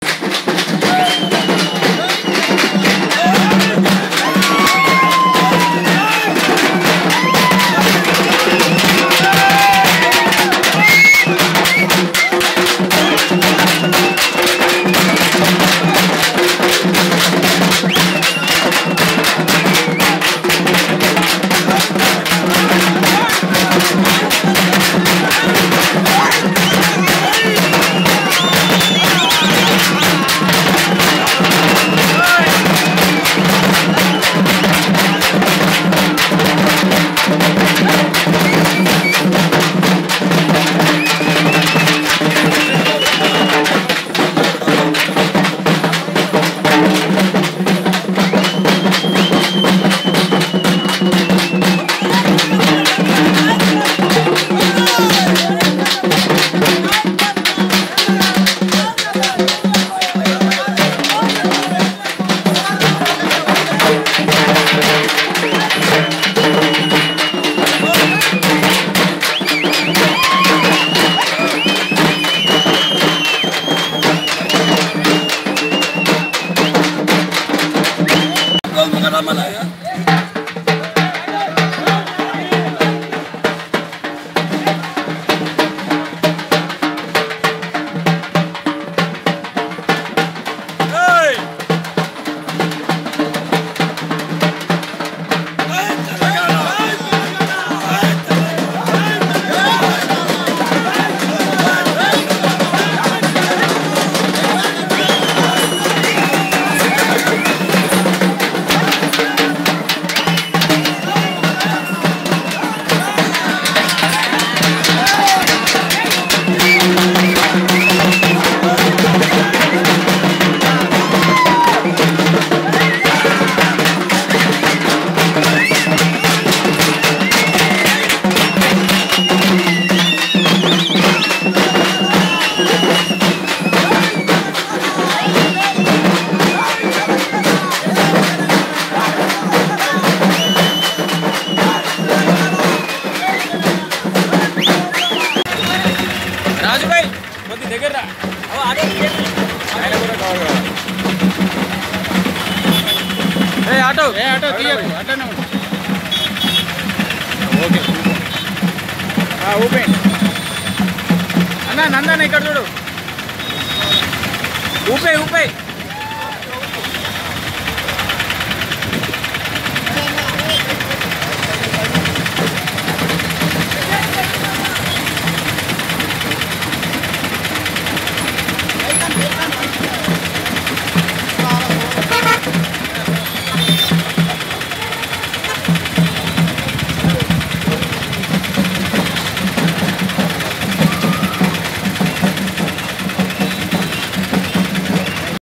డప్పుల హోరుతో శేరిలింగంపల్లి టీఆర్ఎస్ నేతల హంగామా TRS Serilingampally leaders hangama
MLA Arekapurdi Gandhi supporters and TRS Serilingam palli leaders hangama in Hyderabad City limits outcuts